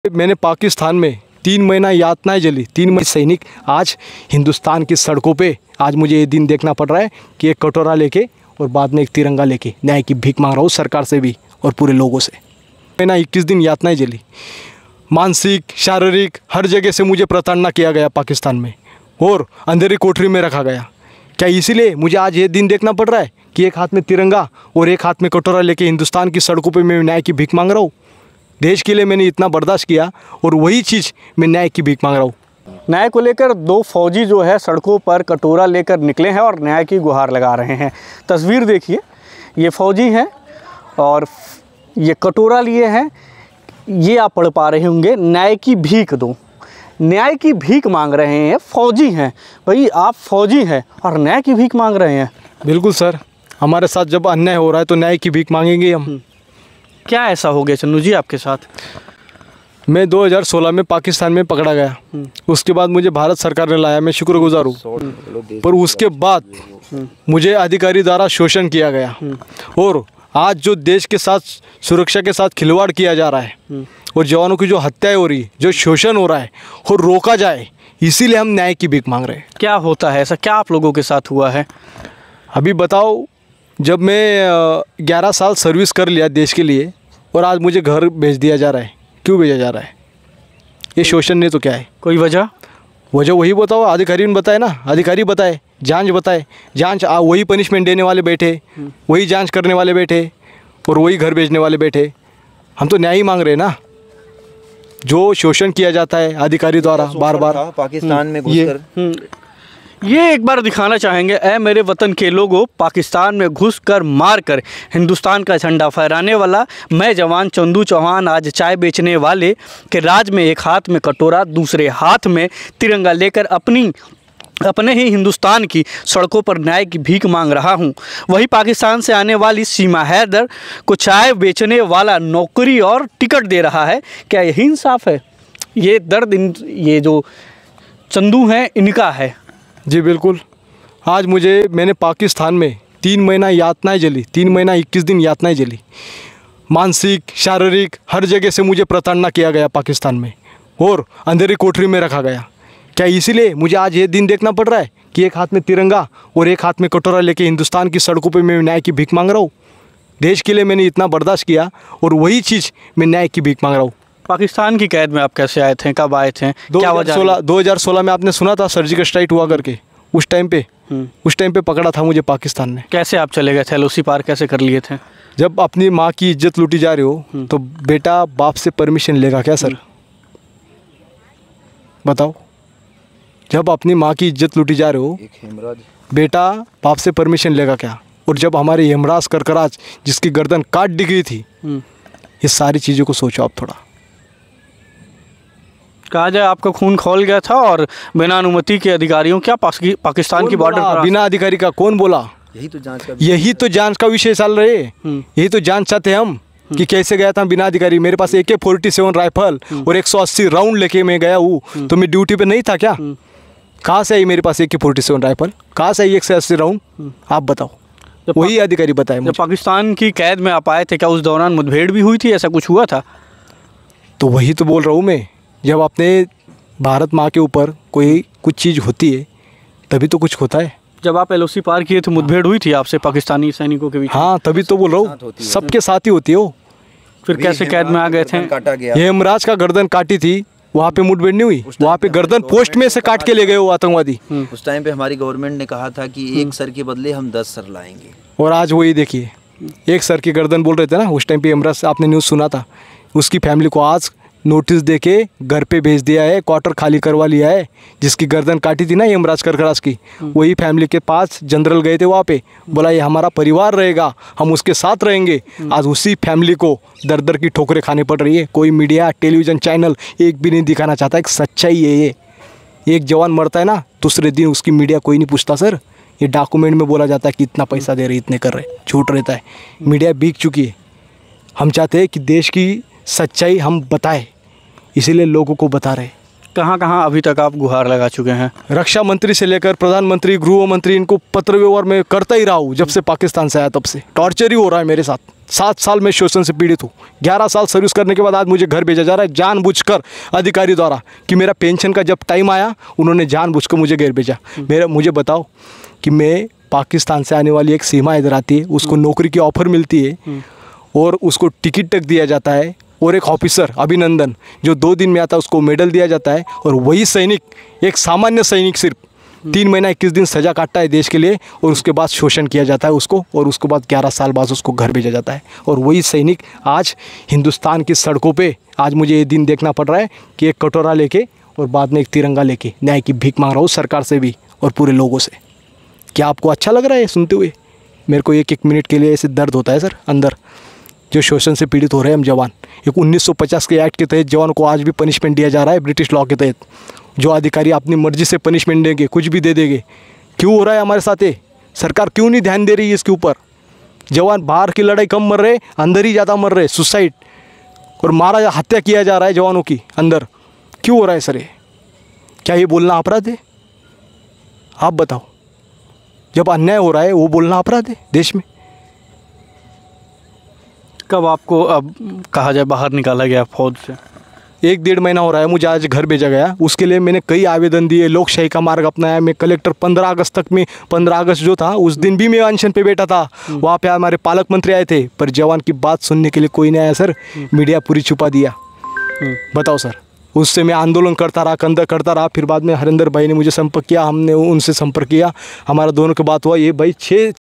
फौजी क्यों मांग रहे हैं भीख, 3 महीने था पाकिस्तान की कैद में
ख़बर और विज्ञापन के लिए सम्पर्क करे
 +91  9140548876